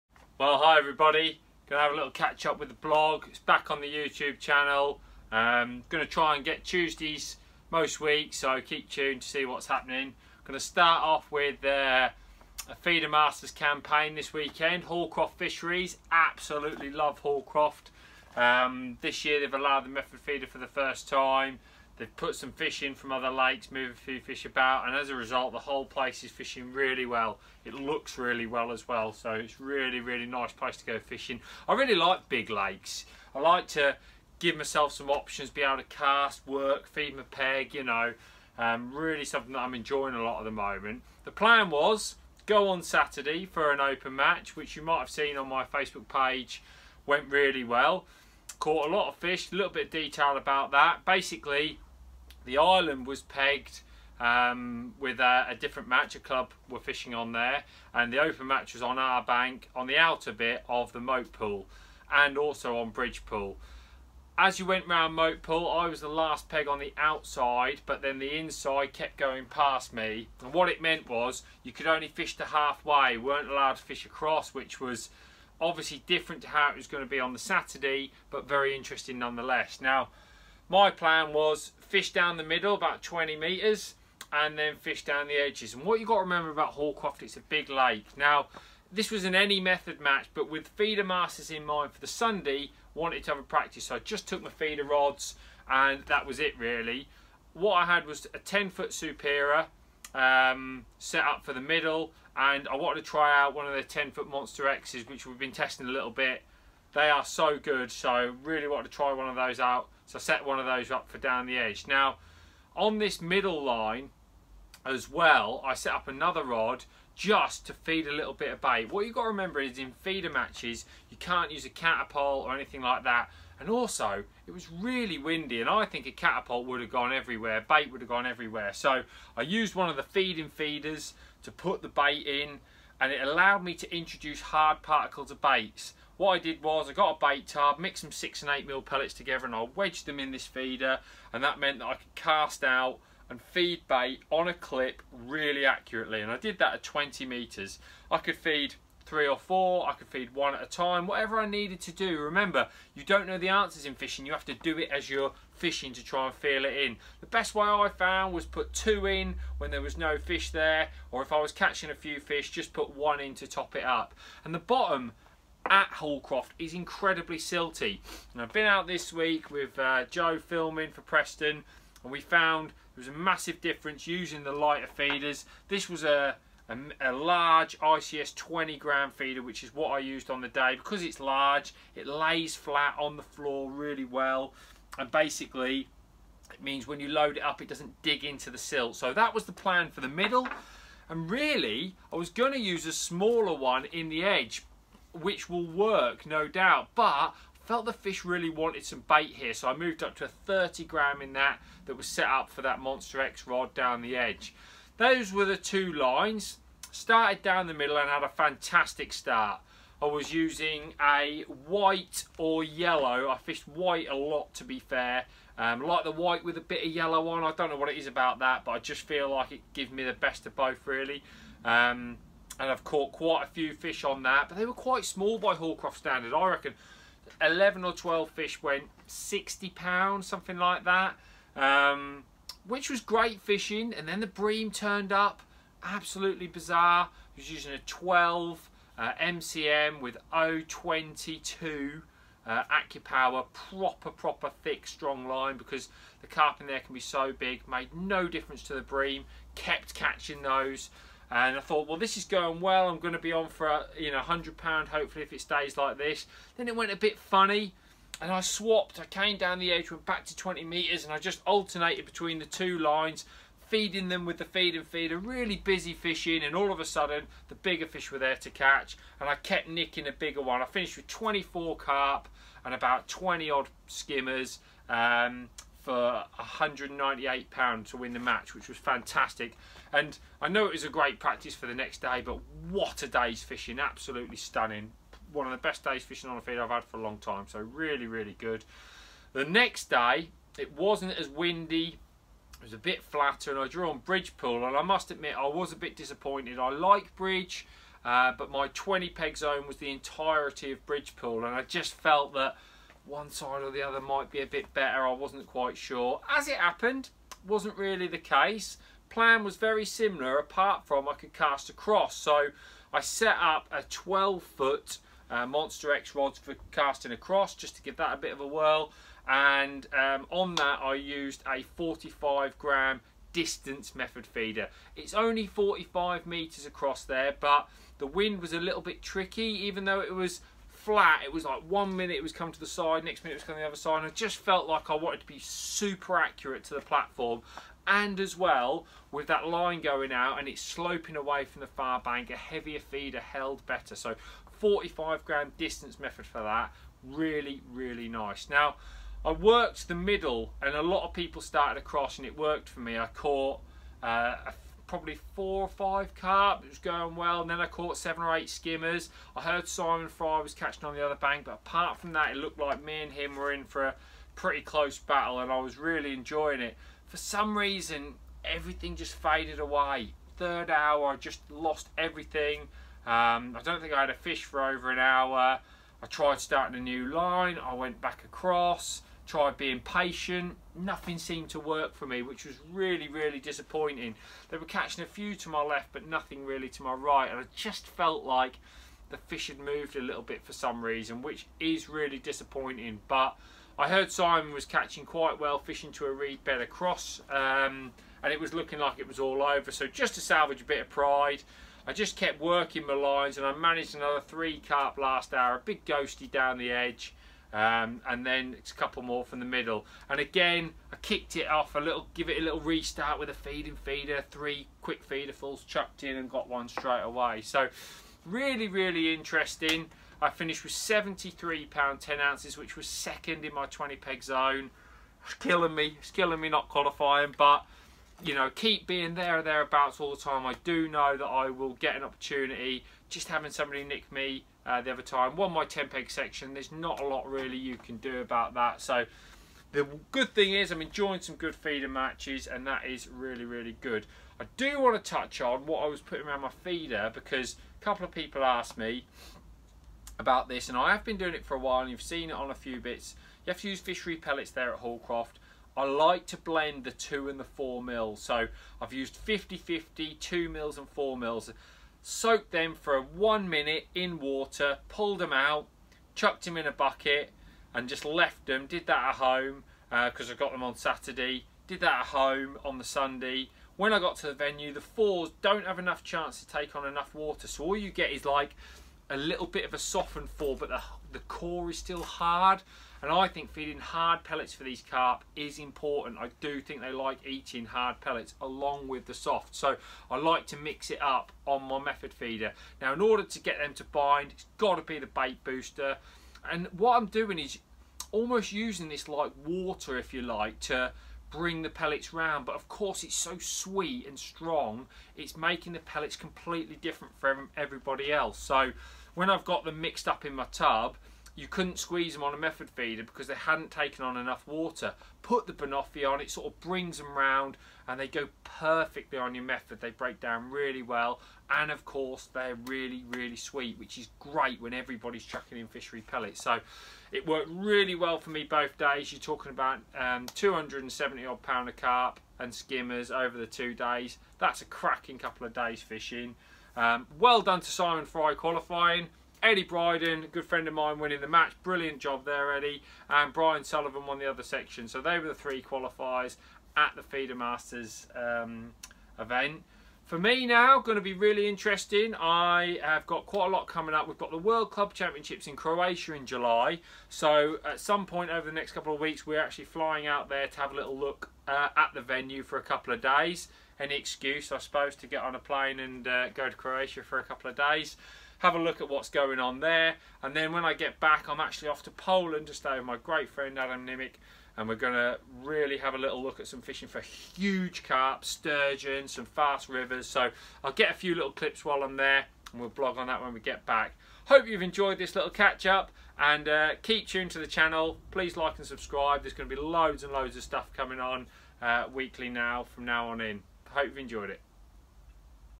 Lee Kerry’s Fishing Vlog - We are back!
Lee starts a new series of blogs with a catch up from his latest Feeder Masters campaign.